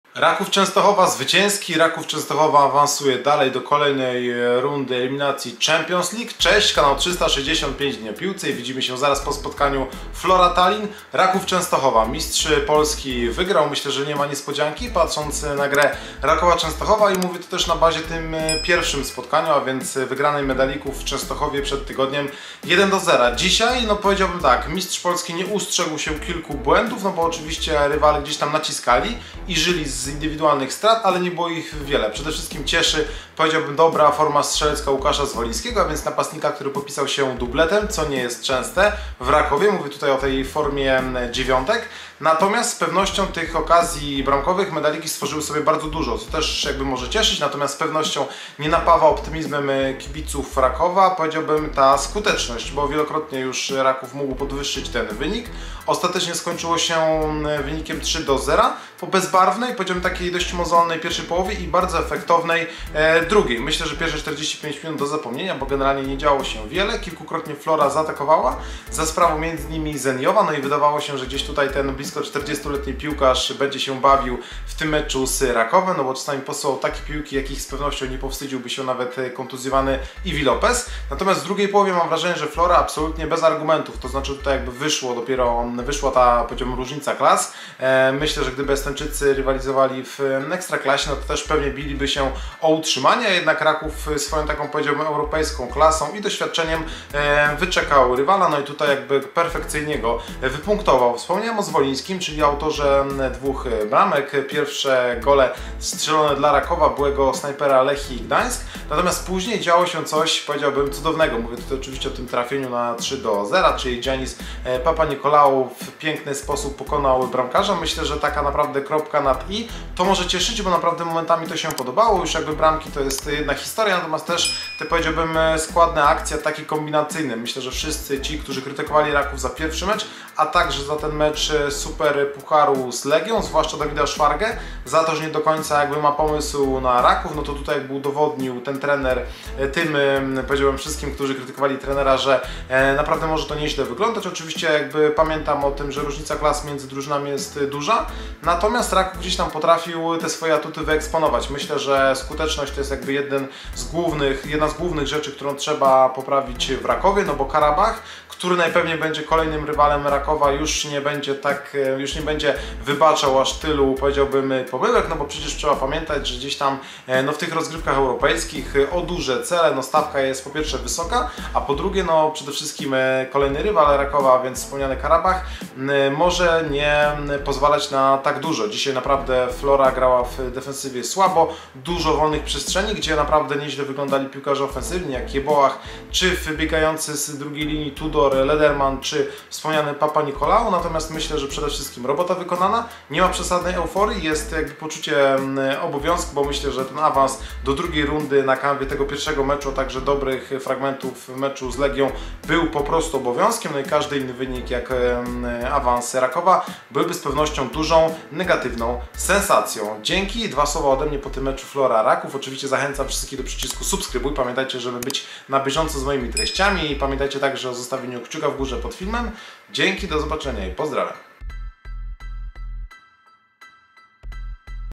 Flora Tallinn 0-3 Raków Częstochowa | Liga Mistrzów 2023/2024 | Robota wykonana. Czas na Qarabağ!
✅Czytaj mnie na Twitterze. Wrzucam tam swoje opinie nt. bieżących tematów

📅 18 lipca 2023, 19:00
🏟  Stadion: A. Le Coq Arena, Tallinn, Estonia.

Flora Tallinn - Raków Częstochowa 0:3 (0:0)

Bramki:
0:1 ⚽ Łukasz Zwoliński 47'
0:2 ⚽ Łukasz Zwoliński 58'
0:3 ⚽ Giannis Papanikolaou 84'

Skład Flora Tallinn:

Evert Grünvald - Michael Lilander, Märten Kuusk, Markkus Seppik (82' Aleksandr Šapovalov) - Kristo Hussar, Konstantin Vassiljev, Martin Miller, Danil Kuraksin (79' Marko Lipp), Rauno Alliku, Henrik Ojamaa - Sten Reinkort (62' Nikita Mihhailov).
Trener: Jürgen Henn.

Skład Rakowa Częstochowa:

Adnan Kovacević - Zoran Arsenić, Milan Rundić, Stratos Svarnas, Fran Tudor (79' Mateusz Wdowiak) - Jean Carlos Silva (79' Deian Sorescu), Giannis Papanikolaou, Ben Lederman, Marcin Cebula (67' Władysław Koczerhin), Bartosz Nowak (67' John Yeboah) - Łukasz Zwoliński (67' Fabian Piasecki).
Trener: Dawid Szwarga.

🟨 Żółte Kartki:
Flora Tallinn - 67' Markkus Seppik.
Raków Częstochowa - 56' Stratos Svarnas.

Sędzia: Marian Alexandru Barbu (Rumunia).